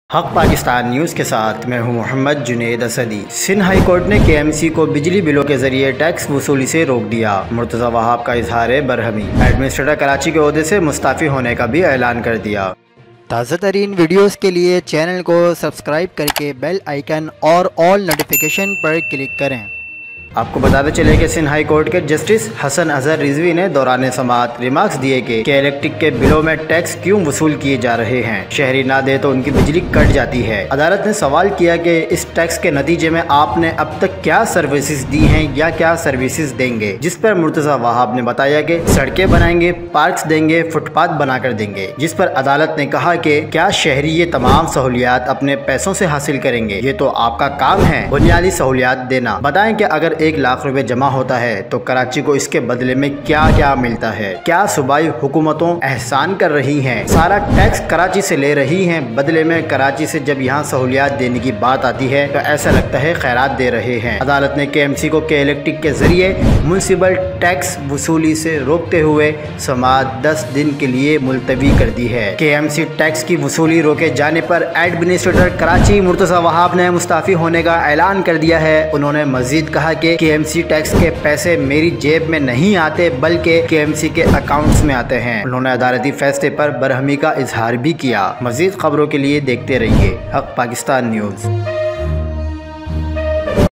Karachi K-Electric Ab Tax Nahi Lega | Administrator Karachi Murtaza Wahab Mustafi Hone Elan KMC News
Good news for Karachi, K Electric will no longer charge tax
The announcement of the resignation of Administrator Karachi Murtaza Wahab
Administrator Karachi Murtaza Wahab ka mustafi hone ka ailan